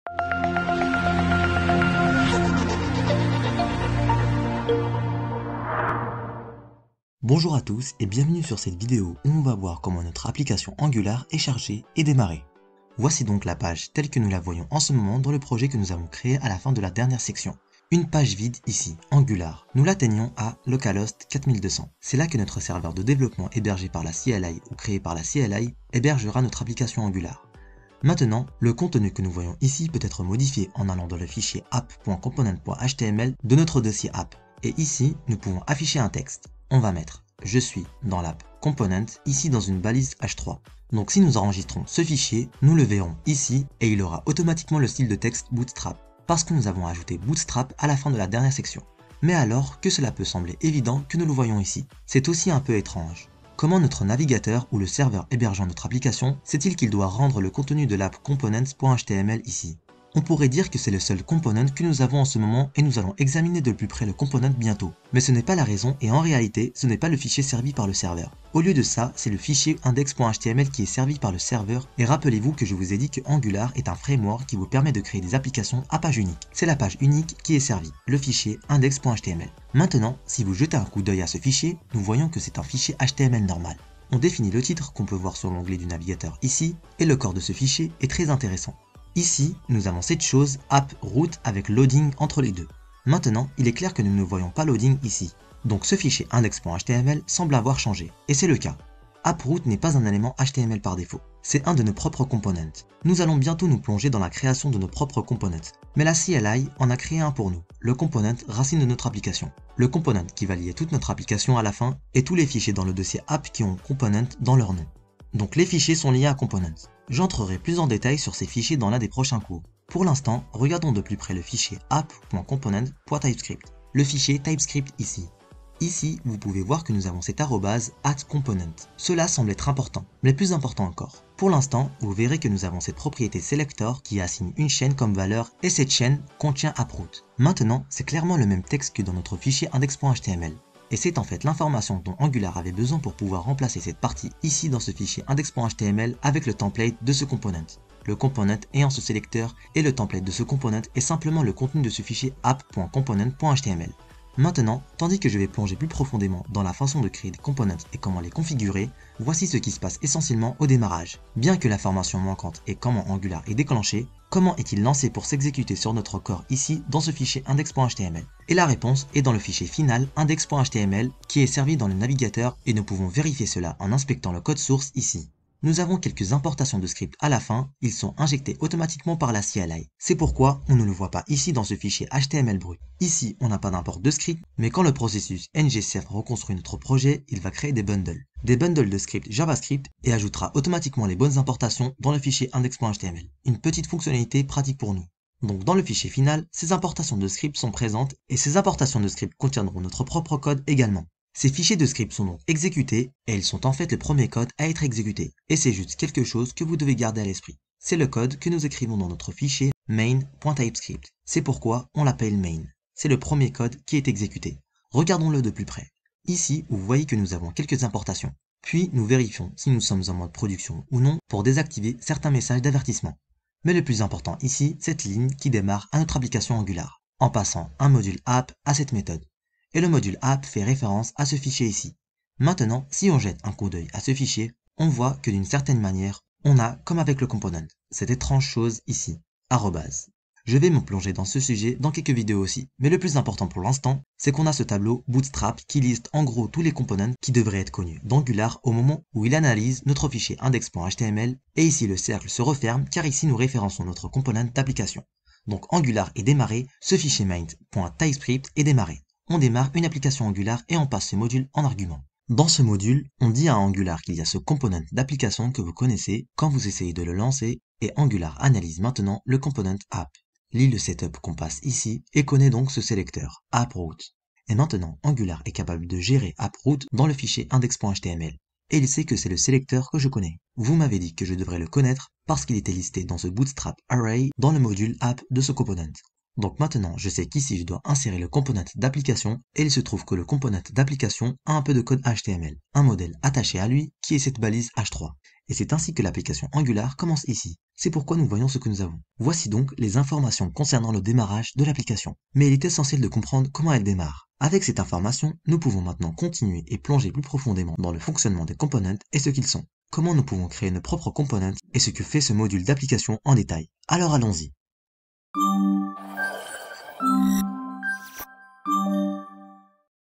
Apprendre Angular 2 2 Comment une application Angular est chargée et démarrer
La vidéo "Apprendre Angular 2.2 : Comment une application Angular est chargée et démarrée" est une formation en français sur le processus de chargement et de démarrage d'une application Angular. La vidéo est animée par un formateur qui explique les différentes étapes du processus de chargement et de démarrage d'une application Angular, en commençant par la phase de chargement des modules, suivie de la création du composant principal et de l'injection des dépendances. Le formateur explique également comment Angular traite les templates HTML et les transforme en vues de l'application, et comment le moteur de rendu d'Angular permet de maintenir la performance de l'application. La vidéo se termine par une démonstration pratique de la création d'une application Angular simple, où le formateur montre comment le processus de chargement et de démarrage est appliqué dans la pratique. La vidéo est d'une durée d'environ 25 minutes et est hébergée sur YouTube.